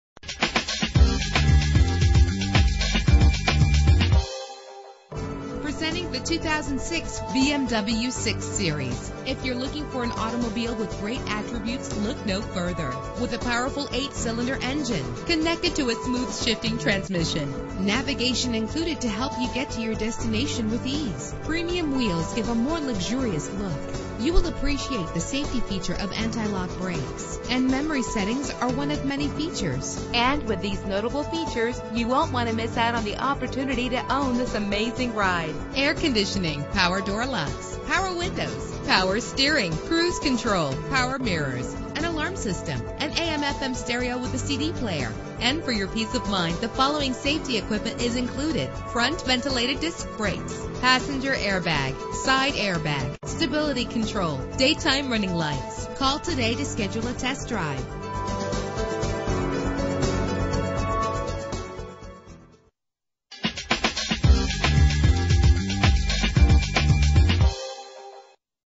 2006 BMW 6 Series - Chandler AZ
Year: 2006
Make: BMW
Model: 6 Series
Trim: 650i
Engine: 4.8 liter 8 cylinder 32 valve
Transmission:
Color: Mineral Silver
Mileage: 74876
Address:
7455 W Orchid Lane